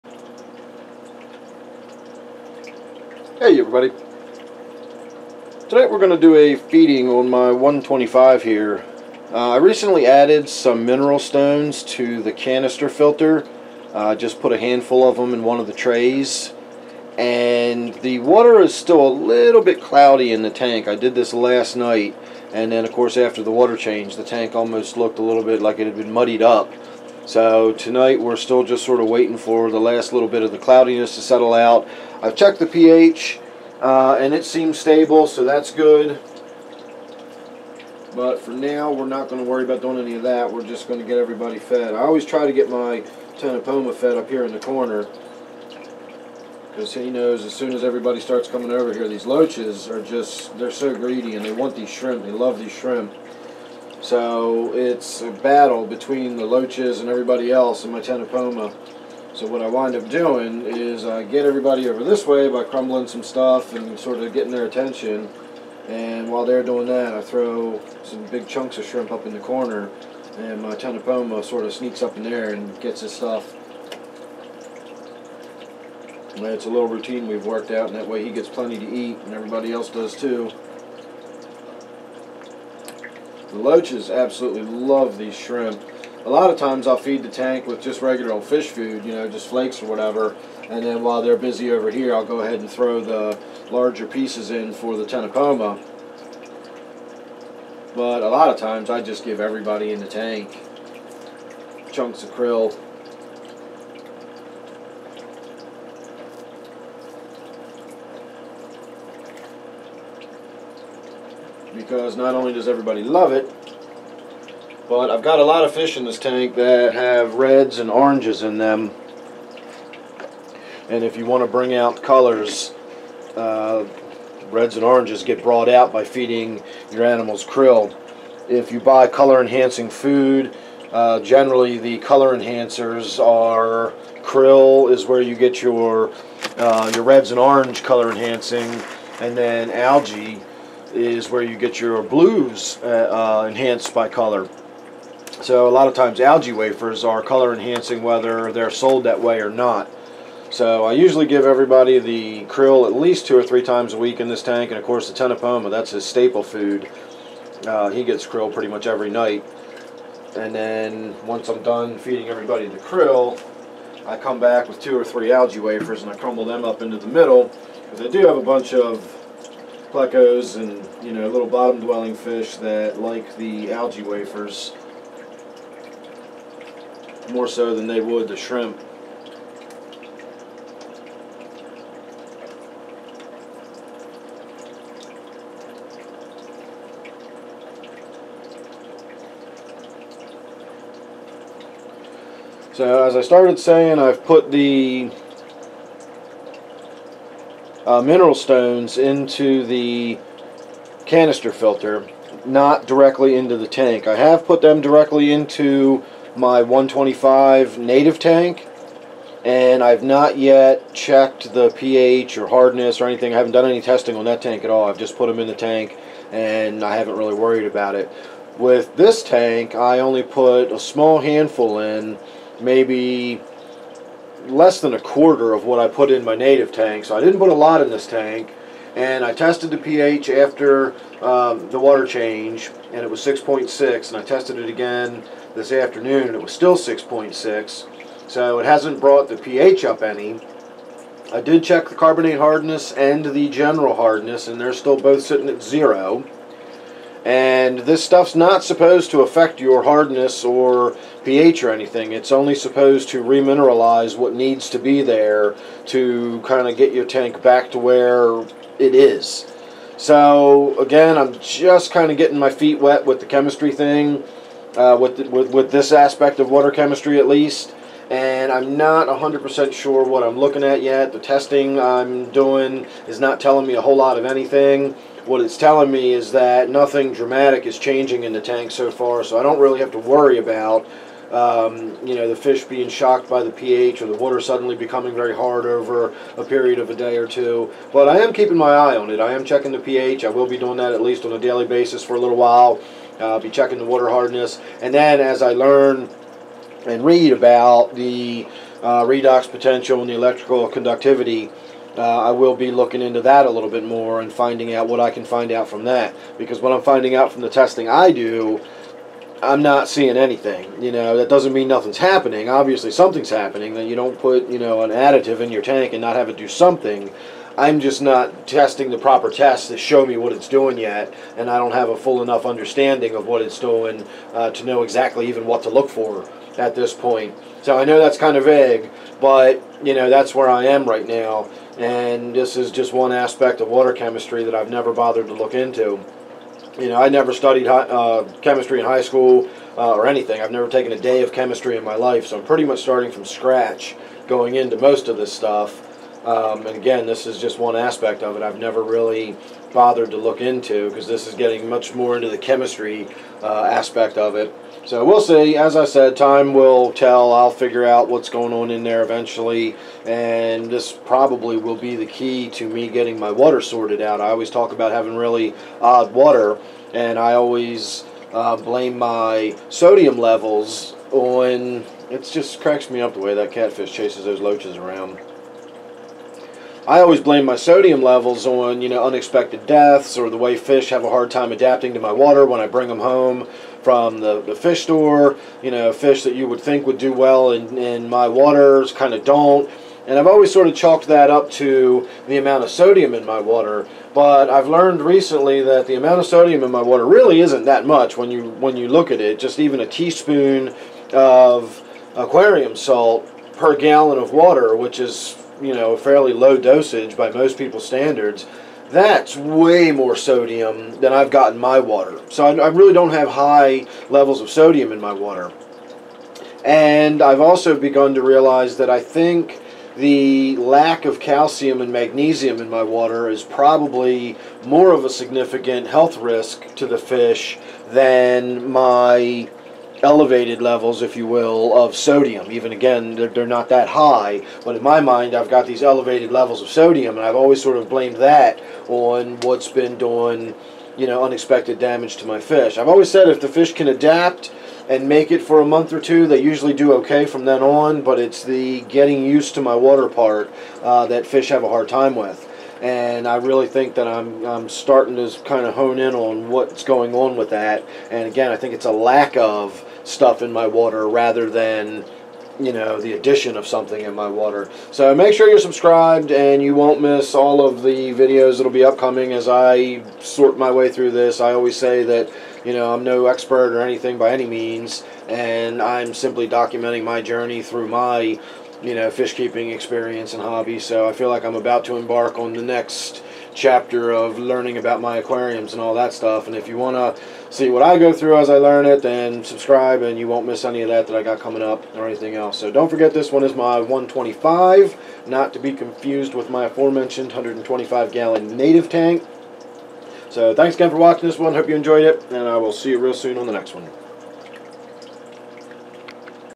A Chat About My Water Chemistry Since Adding Mineral Stones
Newstone Mironekuton natural mineral rock
San Francisco Bay Freeze Dried Krill
Here's a look at my 125 gallon "African" themed planted tank. I have a Ctenopoma acutirostre, Synodontis eupterus, Clown Loaches, plecos, Congo Tetras, Rainbowfish and more.